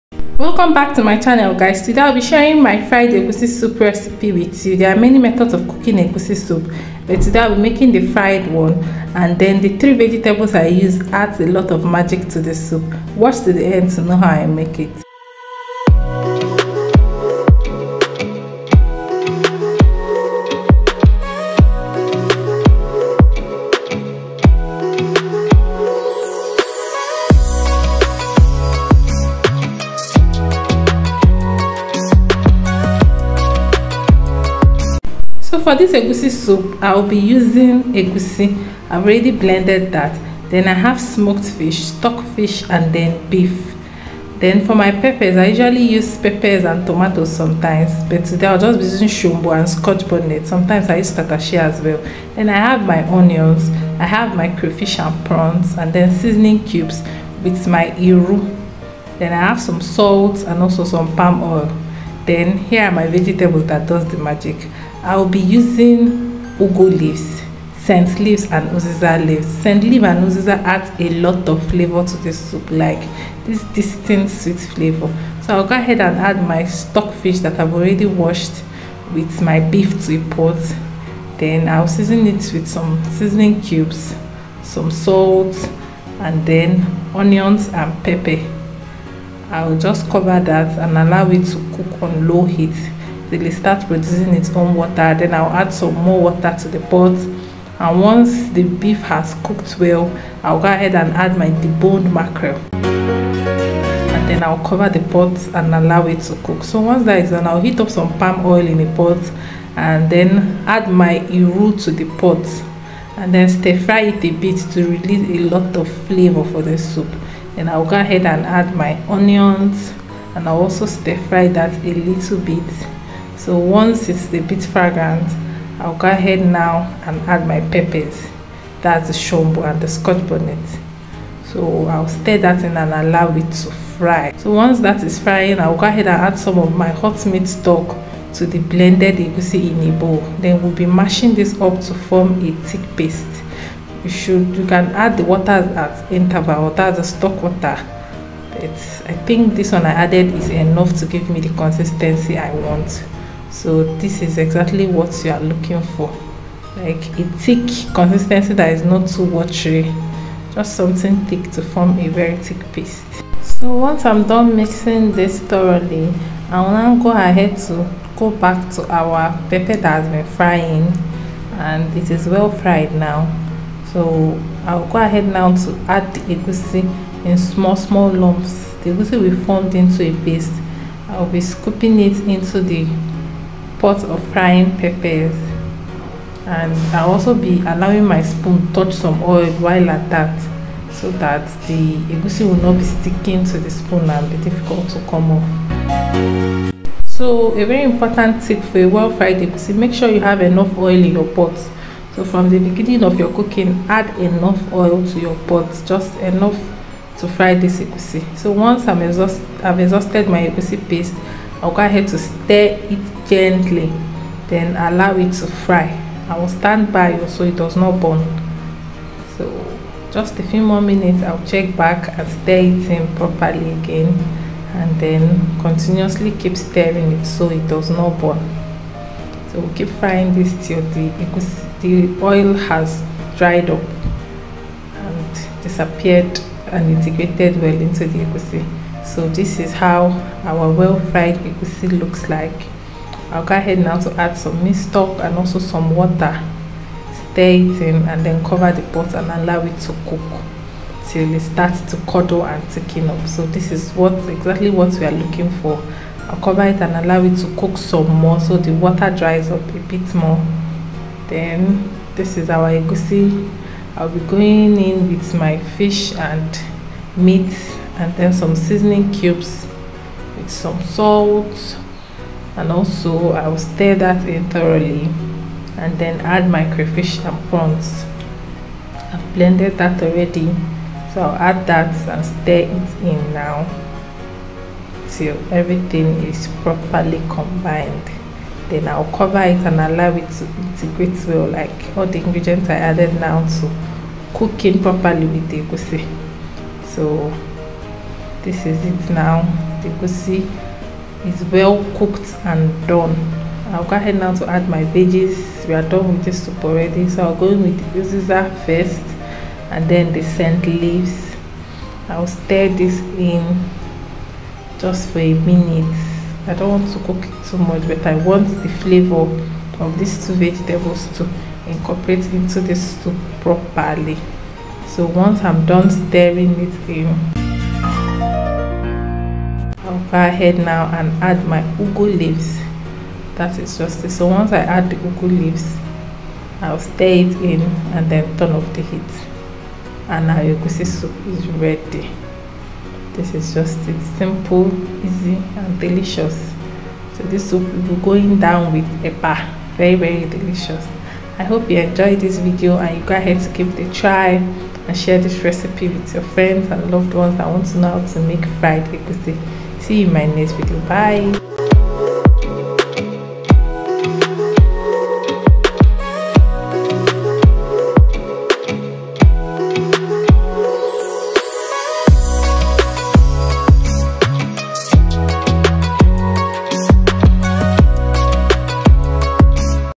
EGUSI SOUP WITH THREE VEGETABLES/FRIED EGUSI SOUP/PARTY EGUSI SOUP/ HOW TO COOK EGUSI SOUP
Fried egusi tastes real good on it's own, but the 3 vegetables I'll be using for this soup will take this Egusi soup to the next level.

For this soup, I'll be using
3cups of Egusi.
Beef
Stock fish
Smoked Mackerel
Shombo
Scotch bonnet peppers
Onions
Iru ( locust beans)
Crayfish
Smoked prawns
Seasoning cubes
Salt
Palm oil
Vegetable; Ugu, Uziza leaves, Scent leaves.

Philippians 4:13